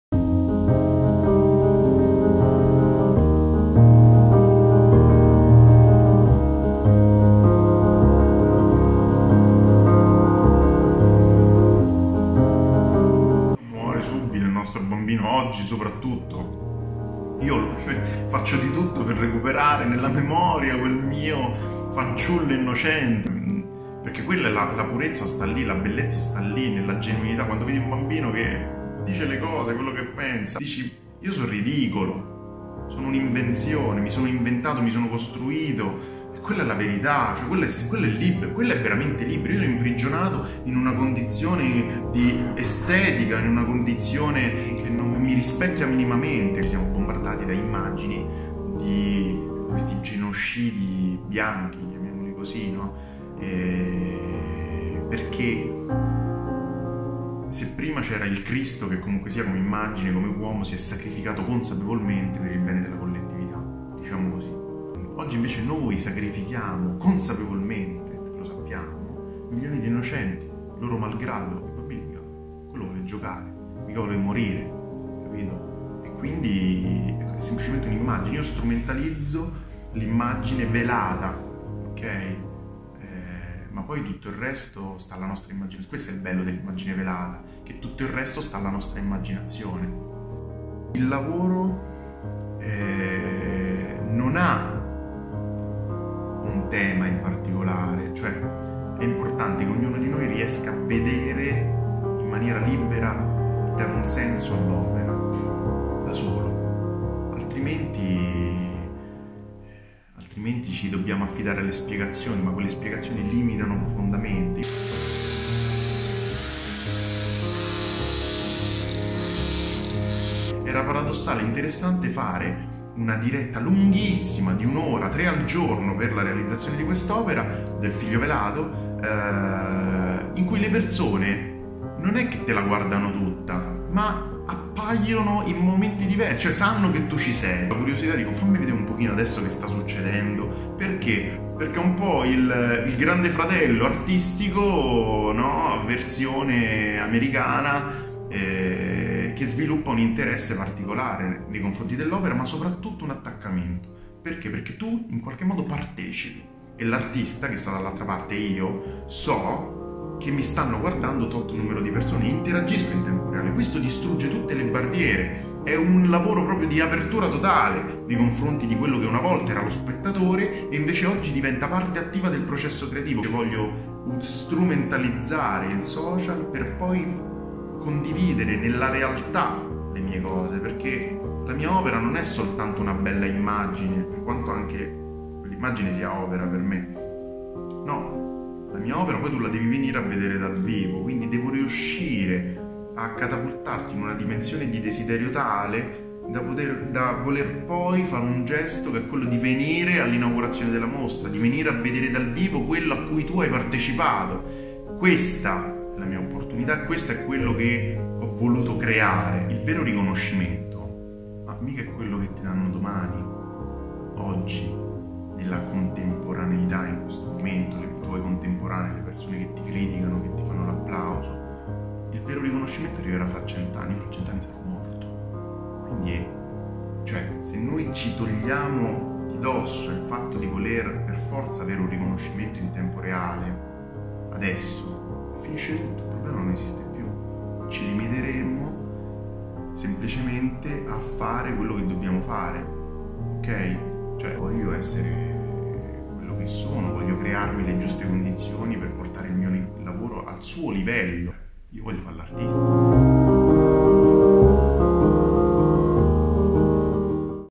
Il Figlio Velato. Intervista allo scultore Jago
“Il Figlio Velato” arriva a Napoli, nel rione Sanità. La Chiesa di San Severo fuori le Mura accoglie l’ultima opera in marmo bianco del giovane artista Jago. Intervista a cura di Giada Giorgi.